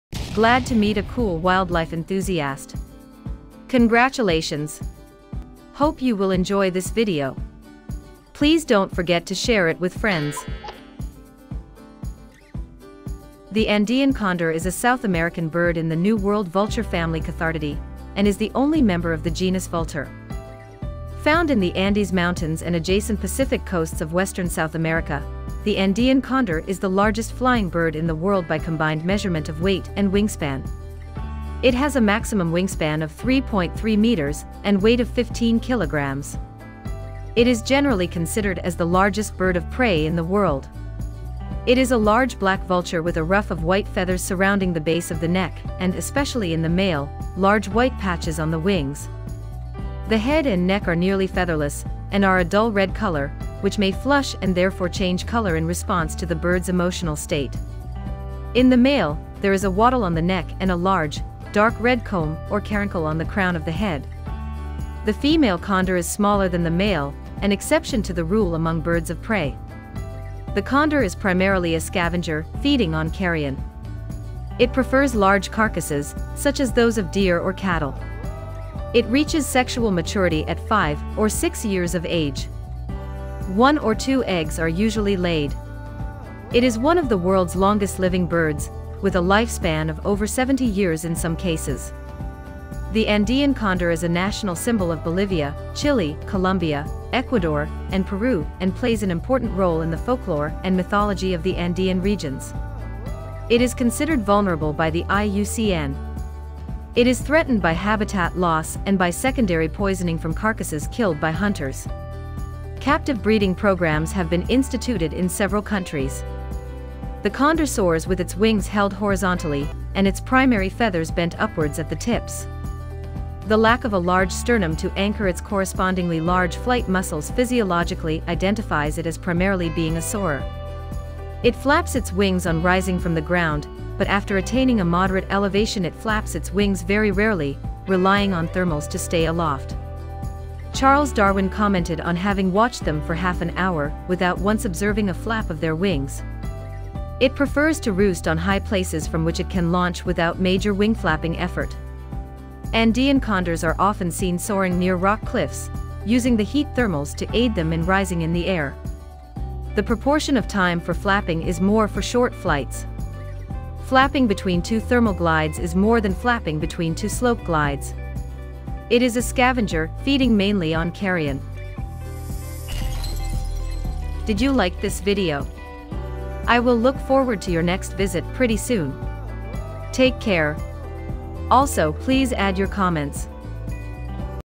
Andean Condor facts 🦅 Vulture Found in Andes mountains 🏔 largest flying bird by weight wingspan 🤩
birddocumentary Andean condor documentary
The Andean condor is a South American bird in the New World vulture family Cathartidae and is the only member of the genus Vultur. Found in the Andes mountains and adjacent Pacific coasts of western South America, the Andean condor is the largest flying bird in the world by combined measurement of weight and wingspan. It has a maximum wingspan of 3.3 meters and weight of 15 kilograms. It is generally considered as the largest bird of prey in the world.
It is a large black vulture with a ruff of white feathers surrounding the base of the neck and, especially in the male, large white patches on the wings. The head and neck are nearly featherless, and are a dull red color, which may flush and therefore change color in response to the bird's emotional state. In the male, there is a wattle on the neck and a large, dark red comb or caruncle on the crown of the head. The female condor is smaller than the male, an exception to the rule among birds of prey.
The condor is primarily a scavenger, feeding on carrion. It prefers large carcasses, such as those of deer or cattle. It reaches sexual maturity at five or six years of age. One or two eggs are usually laid. It is one of the world's longest-living birds, with a lifespan of over 70 years in some cases.
The Andean condor is a national symbol of Bolivia, Chile, Colombia, Ecuador, and Peru and plays an important role in the folklore and mythology of the Andean regions. It is considered vulnerable by the I-U-C-N. It is threatened by habitat loss and by secondary poisoning from carcasses killed by hunters. Captive breeding programs have been instituted in several countries.
The condor soars with its wings held horizontally and its primary feathers bent upwards at the tips. The lack of a large sternum to anchor its correspondingly large flight muscles physiologically identifies it as primarily being a soarer. It flaps its wings on rising from the ground, but after attaining a moderate elevation it flaps its wings very rarely, relying on thermals to stay aloft. Charles Darwin commented on having watched them for half an hour without once observing a flap of their wings. It prefers to roost on high places from which it can launch without major wing-flapping effort. Andean condors are often seen soaring near rock cliffs, using the heat thermals to aid them in rising in the air. The proportion of time for flapping is more for short flights. Flapping between two thermal glides is more than flapping between two slope glides.
It is a scavenger, feeding mainly on carrion.